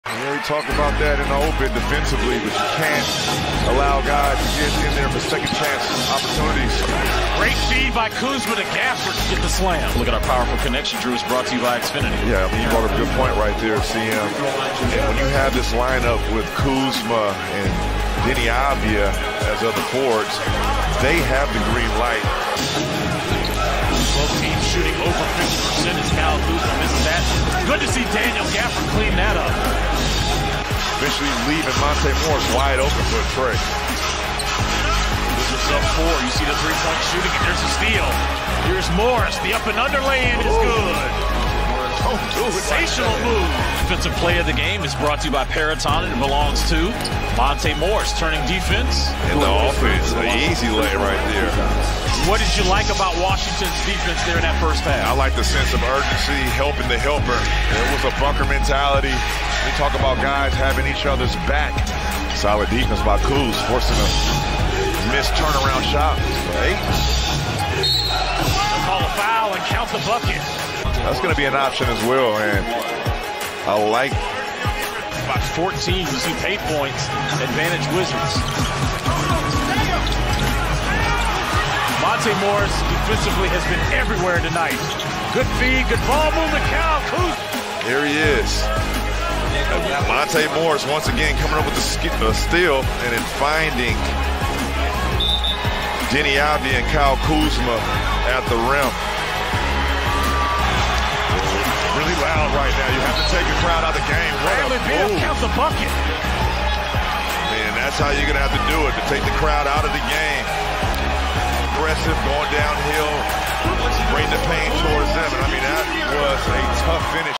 Highlights: Washington Wizards at Phoenix Suns - 12/20/22
Highlights from the Wizards 113-110 victory over the Suns on Tuesday night in Phoenix.